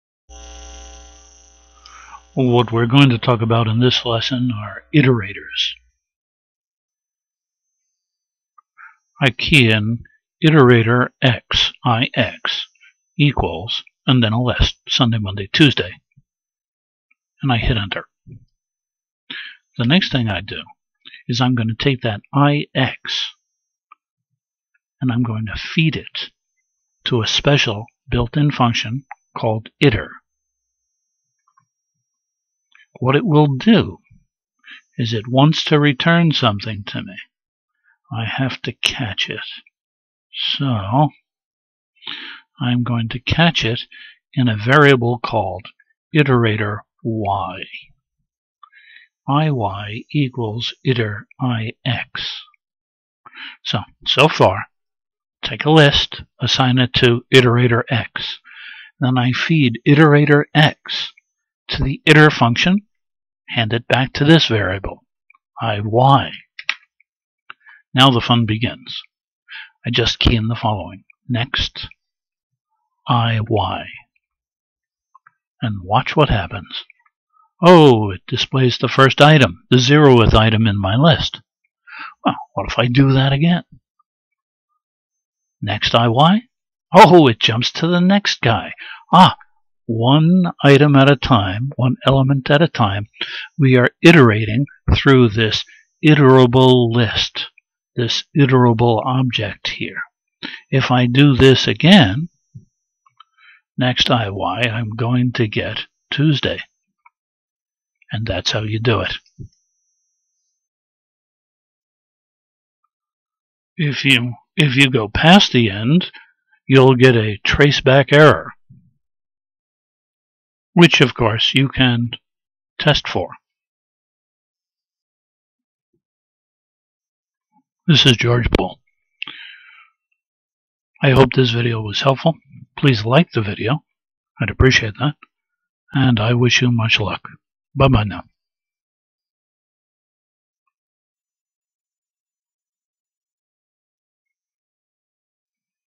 Python Iterators Made Super Easy
I make list, hand the list variable over to iter, get the return value and hand that to next( ) who steps one item at a time through my list.